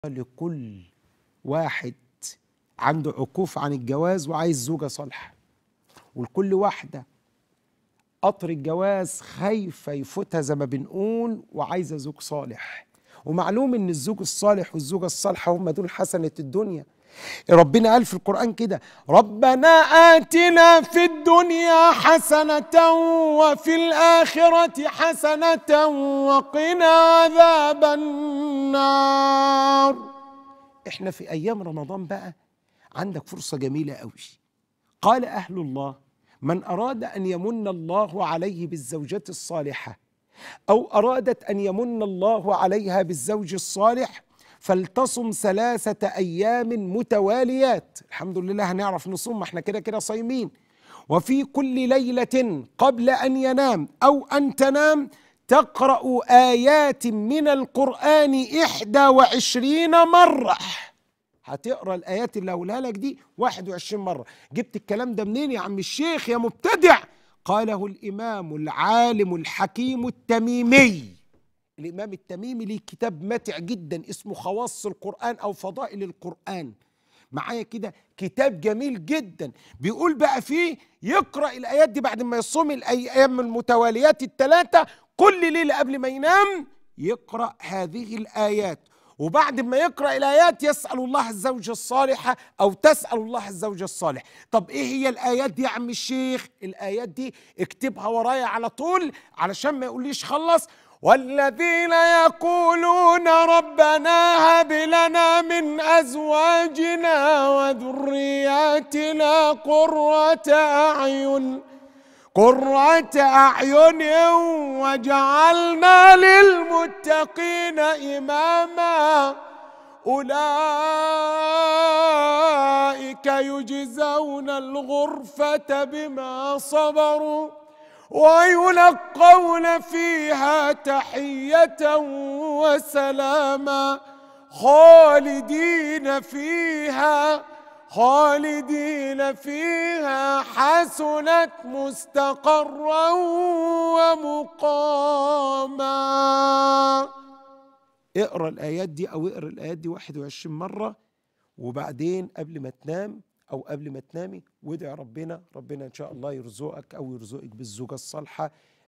هدايا رمضان | من قرأ تلك الآيات يرزقك اله بالزوجة الصالحة
هدايا رمضان 2023
هدايا شهر رمضان 2023 من الشيخ محمد أبو بكر، تضم اشهر الأدعية والآيات المؤثرة في القرآن الكريم.